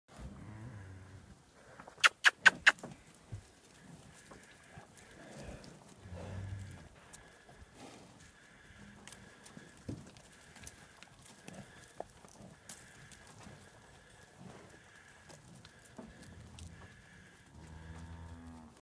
The Cattle Range Listing: # 109D134
8 Angus Cross Bred Heifers... South Central OK
For Sale on The Cattle Range...